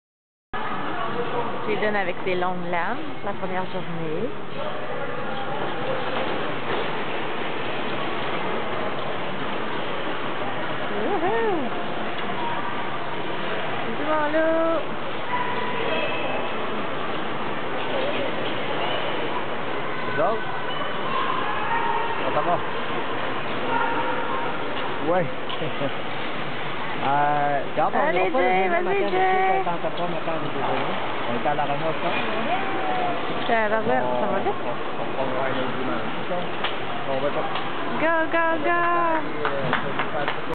Jayden avec longues lames pour la premiere fois.
J'te dis qu'il était fier...mais ma caméra à lâché avant la fin de la course :S